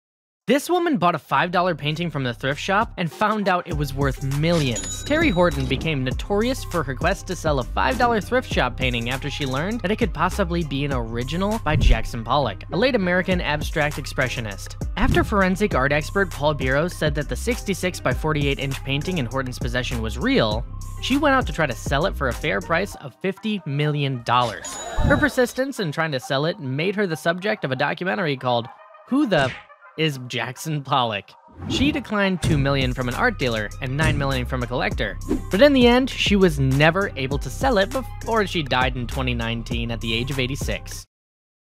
This Woman Bought a $50 Million Painting for $5!
Woman tried turning $5 into $50 million!

The resources and information on this YouTube channel are only meant to be used for educational and informational purposes.